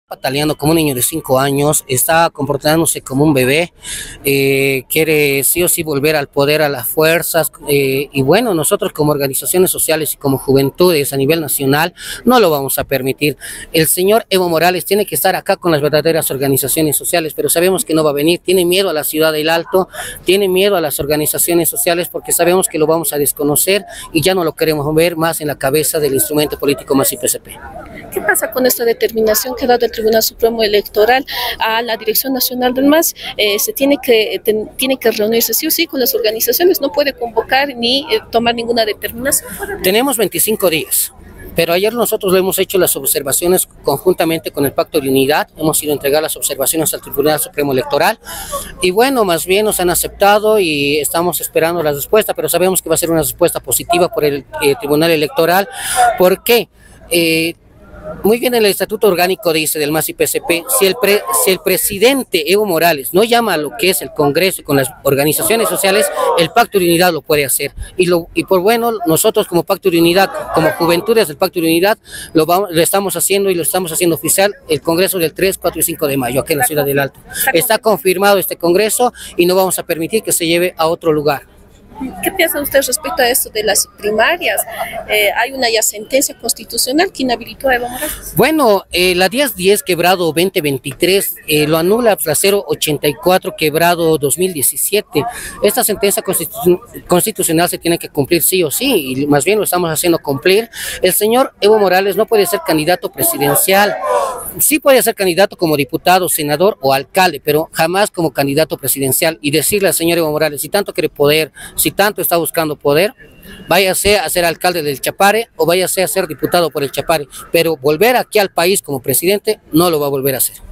Presidente de Juventudes Única de Bolivia ratifica congreso del MAS y recuerda inhabilitación de Evo
📌 Brayan Cuba Castillo, presidente de la Federación de Juventudes Única de Bolivia, reafirmó que las organizaciones subsanaron las observaciones del Tribunal Supremo Electoral (TSE) y confirmó la realización del Congreso del MAS en mayo. Además, recordó que Evo Morales está inhabilitado como candidato presidencial, según una sentencia del Tribunal Constitucional Plurinacional.